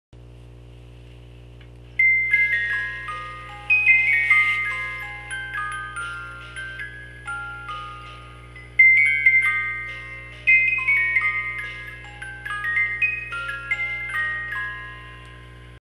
Steinbach Germany Antique Car music box coin bank Thorens Video
This is a vintage mid century musical coin bank in the shape of a an antique jalopy automobile.  It has cloth fringe around the roof, metal fenders, and rubber "Dunlop" tires. On the front is a hand crank with a red plastic handle which turns but serves no functional purpose. It features a Thorens 18 note musical movement made in Switzerland.  The lock on the bottom is marked "Foreign" and comes with the original key(rare). The manufacturer's/tune labels and the horn are missing.  Otherwise it is in excellent cosmetic condition.  I can make another horn but do not have an example of this particular model. There is a stop start shift lever on the driver's side of the car.  Push it forward to start the music and the car moving (car must be on a flat level surface to work). It will move along as it plays. SOLD! To see other music boxes currently for sale